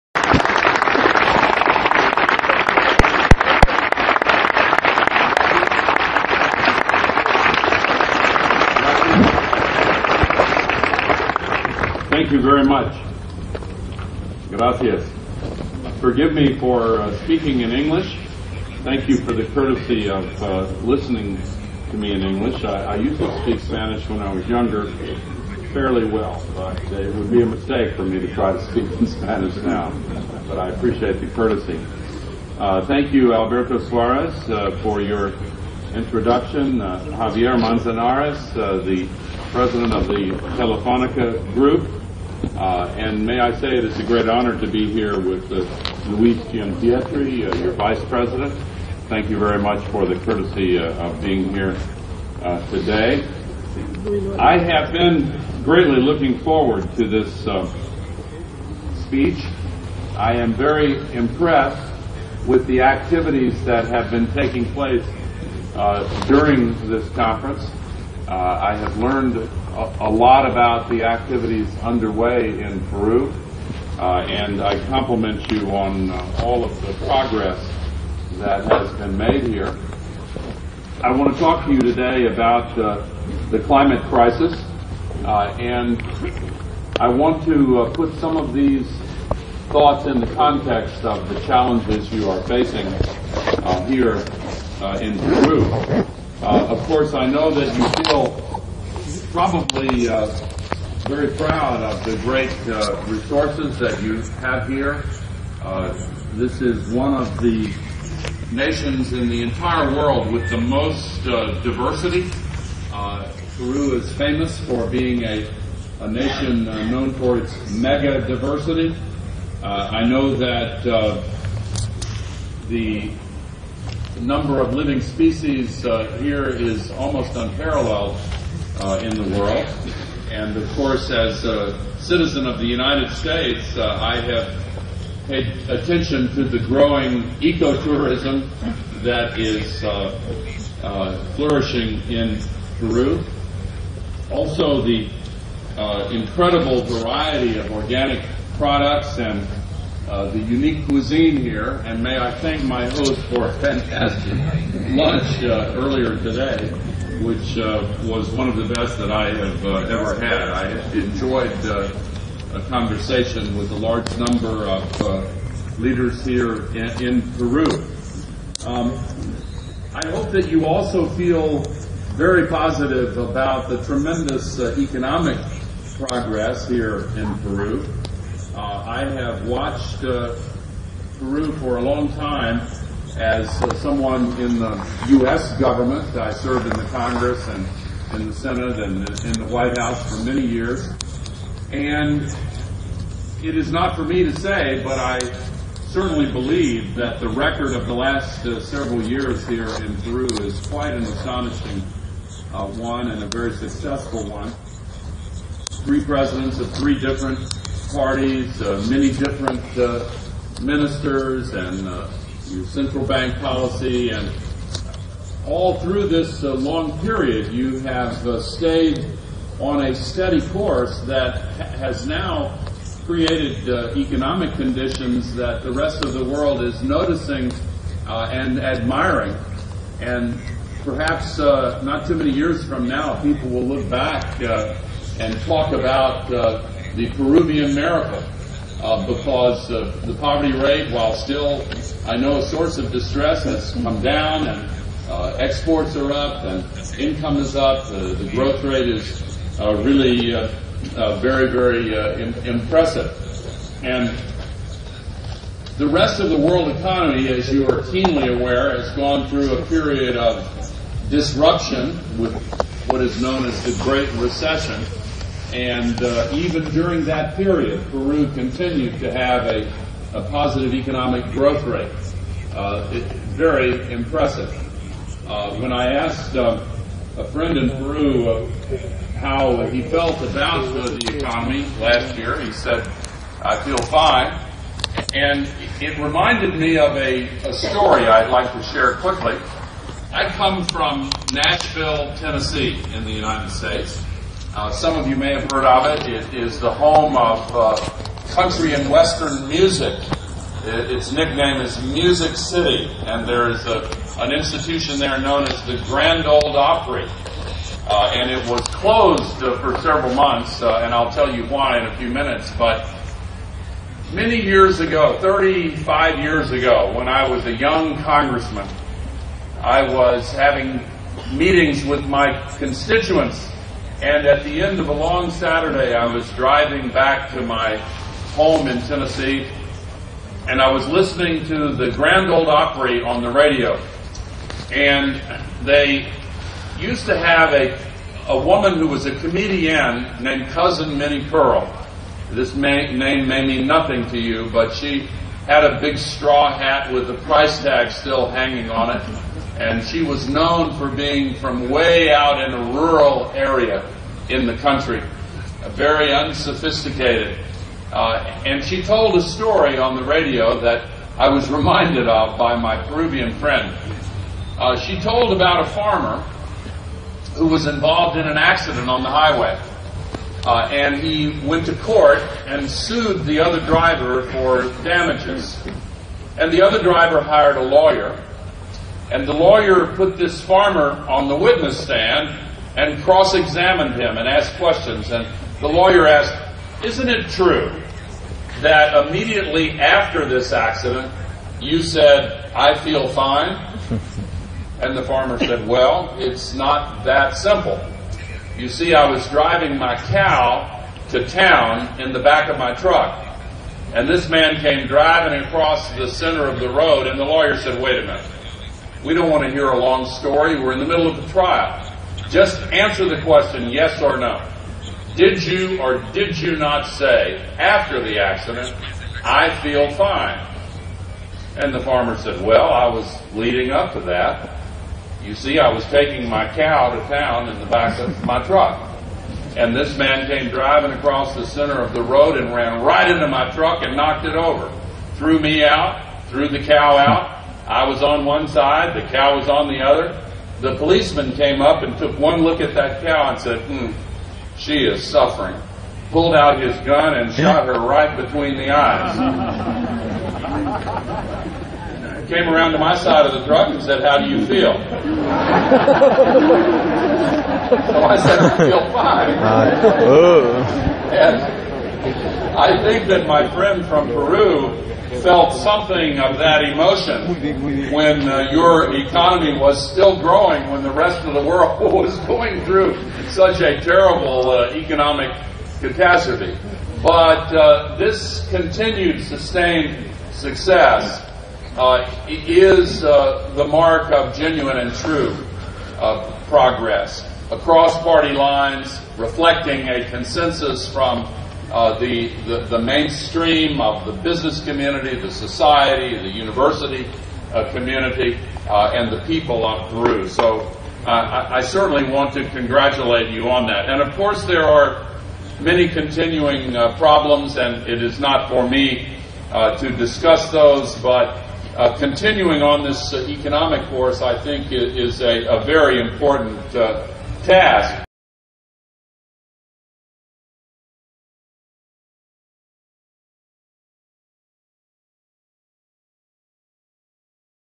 Al Gore PERU 1 Introduction about current situation in Peru
Al Gore's lecture in Lima - October 13th 2010